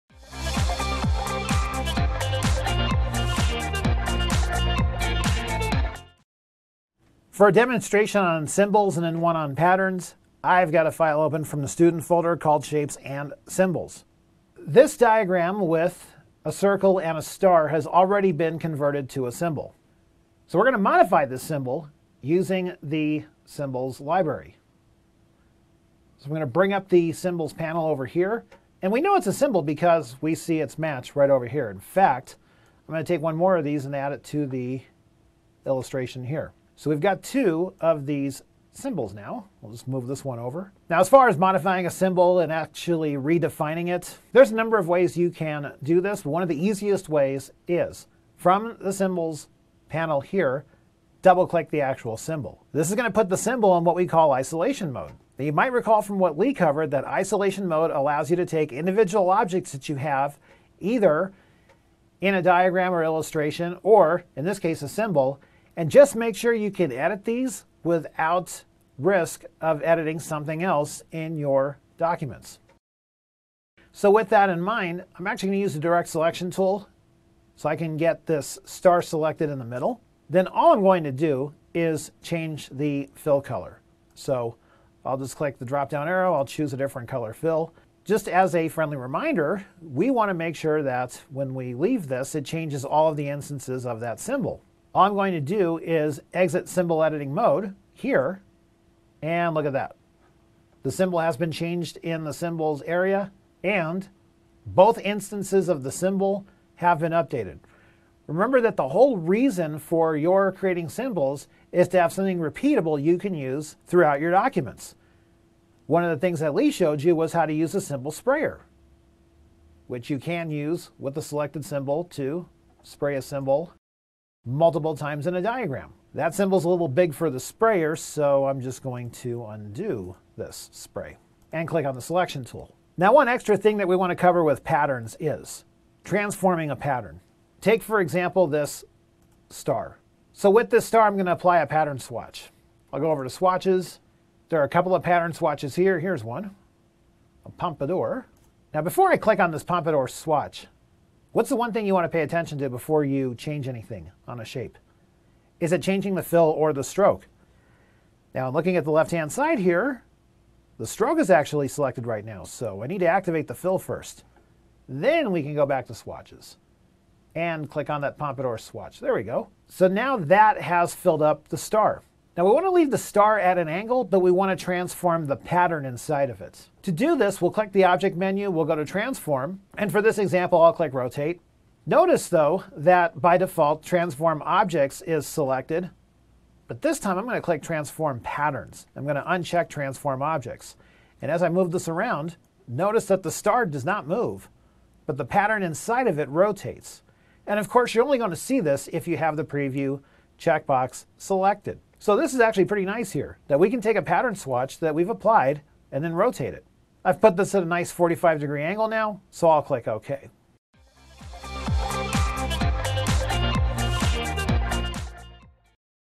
Brushes, Symbols, Graphic Styles, and Patterns - Illustrator CC 2015 tutorial
In this clip from LearnKey's Illustrator CC 2015 Test Prep course, expert Jason Manibog demonstrates how to change the appearance of objects in Adobe Illustrator.